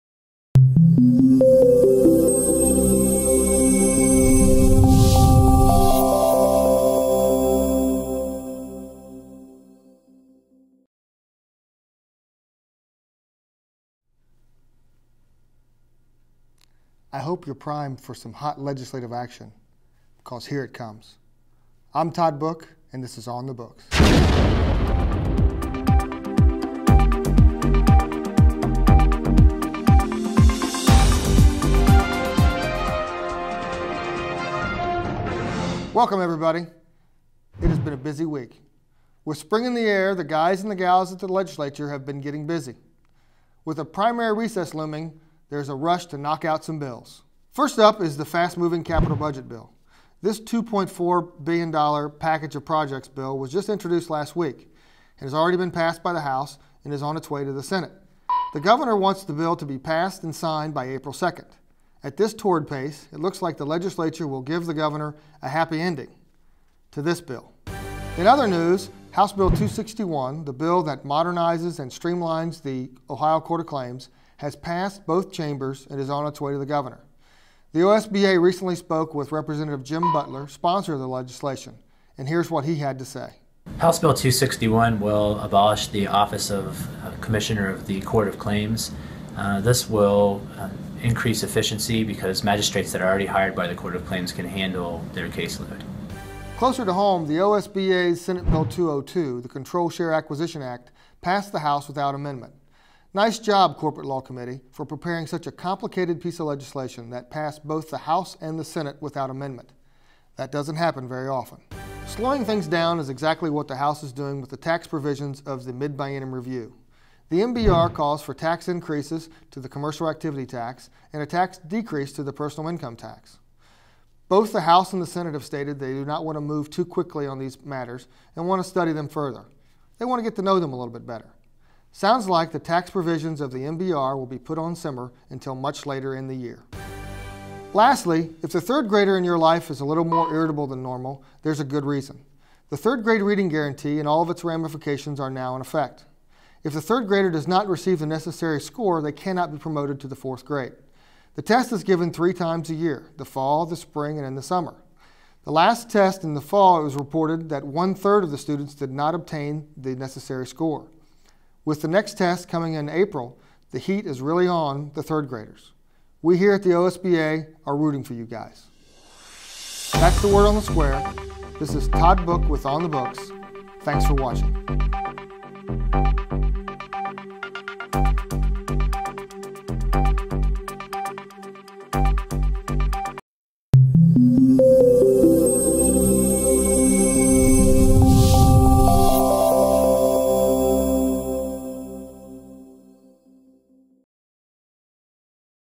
On The Books with Todd Book EP. 4
In this episode of On The Books, Todd discusses House Bill 261, Senate Bill 202, and more, State Rep. Jim Butler even drops by, check it out.